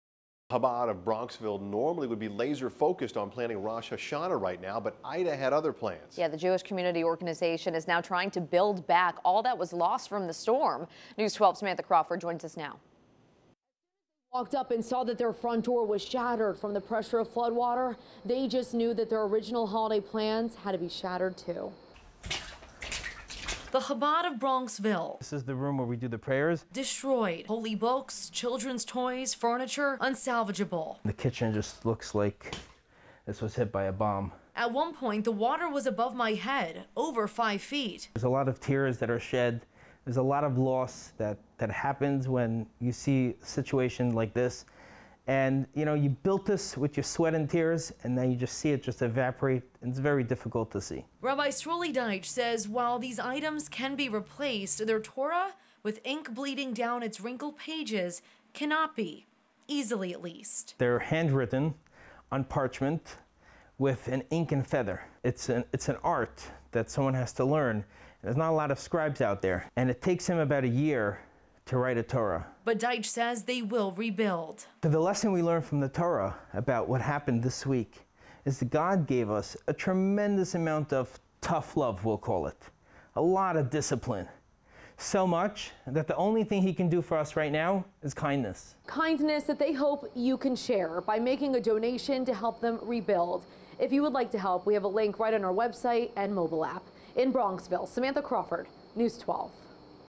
Chabad of Bronxville tries to build back all that was lost as a result of Ida
The Jewish community organization is now trying to build back all that was lost from the historic storm.